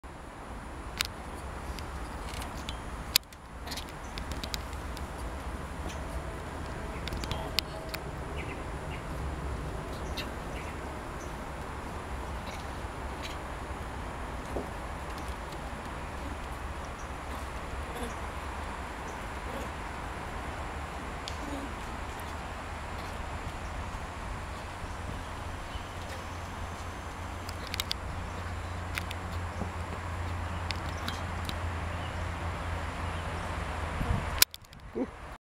MacRitchie Reservoir Park Singapore - Funny Monkeys
These monkeys are Long-tailed Macaque monkeys